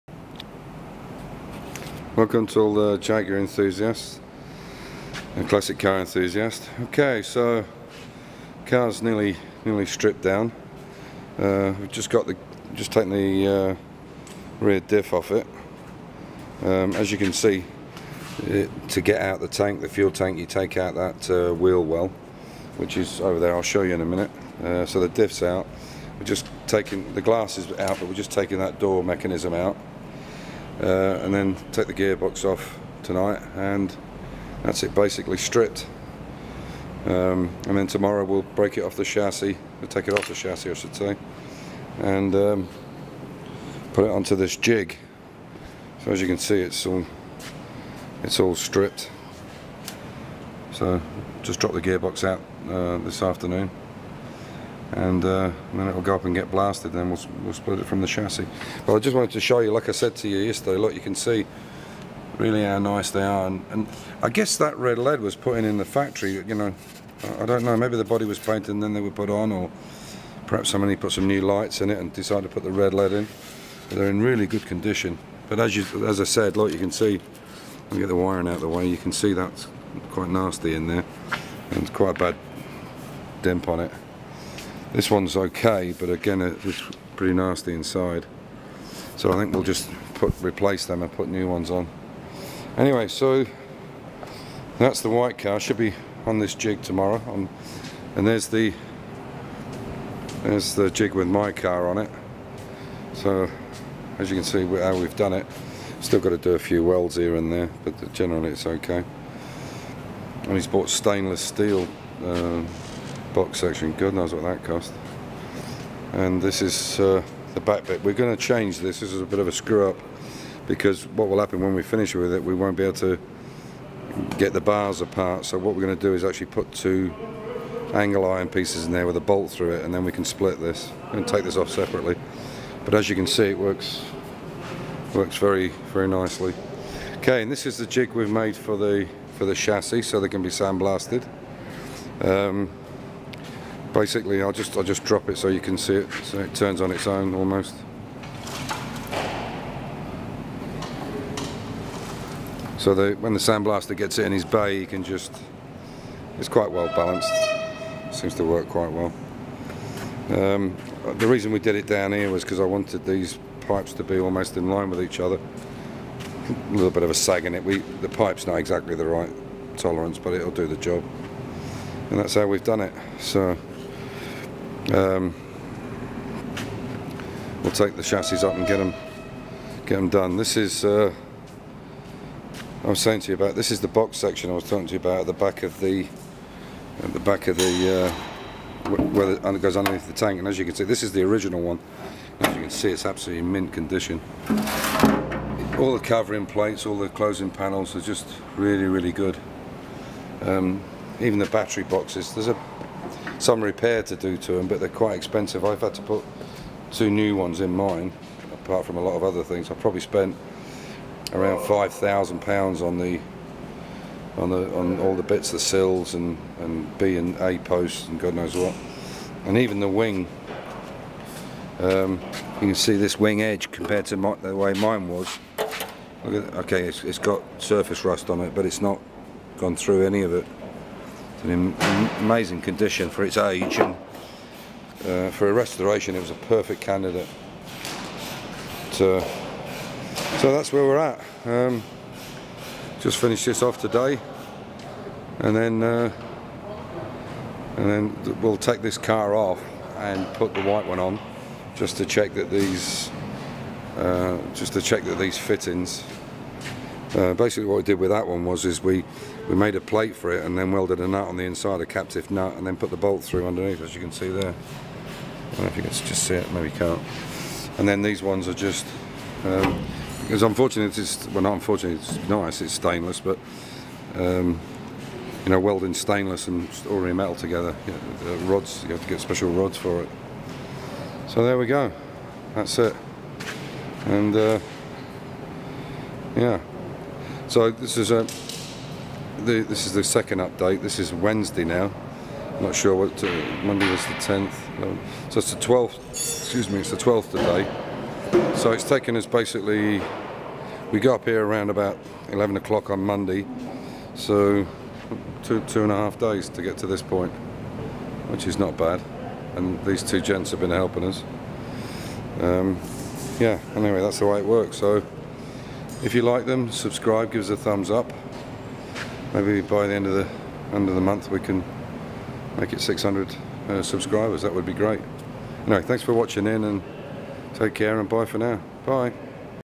jaguar XK body & Jigs day 3
The strip down of the white car is going better than i expected and it looks like we could be finished by Thursday night both the chassis and body jig have worked our well. please contact me with any comments on